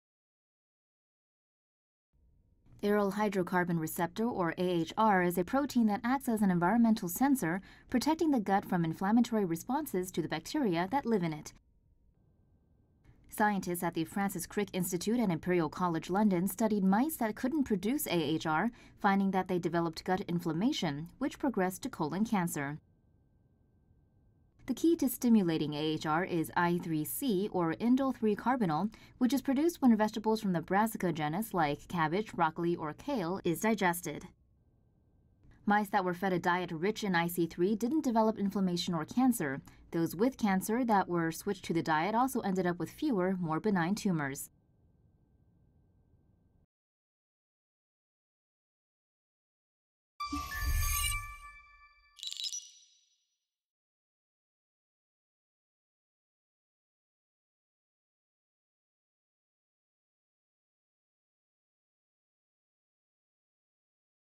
Eating certain vegetables can help prevent colon cancer
New research from the Francis Crick Institute and Imperial College London is offering concrete evidence that eating certain veggies can not only keep the bowels healthy — it also protects from cancer.

According to the study published in the journal Immunity, aryl hydrocarbon receptor or AhR is a protein that acts as an environmental sensor, protecting the gut from inflammatory responses to the bacteria that live in it.

Scientists studied mice that couldn't produce AhR, finding that they developed gut inflammation, which progressed to colon cancer.

The key to stimulating AhR is I3C or indole-3-carbinol, which is produced when vegetables from the Brassica genus like cabbage, broccoli, or kale, is digested.

Mice that were fed a diet rich in I3C didn't develop inflammation or cancer. Those with cancer that were switched to the diet also ended up with fewer, more benign tumors.

The team now wants to see if the veggies have the same effect on people. They're hoping to do further experiments in organoids made from biopsies of the human gut, and eventually go on to human trials.

RUNDOWN SHOWS:
1. Depiction of AhR protecting the gut from inflammation
2. Depiction of mice with no AhR developing colon cancer
3. Depiction of indole-3-carbinol found in vegetables of the Brassica genus
4. Depiction of I3C rich diet reducing inflammation, cancer

VOICEOVER (in English):

"Aryl hydrocarbon receptor or AhR is a protein that acts as an environmental sensor, protecting the gut from inflammatory responses to the bacteria that live in it."

"Scientists at the Francis Crick Institute and Imperial College London studied mice that couldn't produce AhR, finding that they developed gut inflammation, which progressed to colon cancer."

SOURCES:
Imperial College London, BBC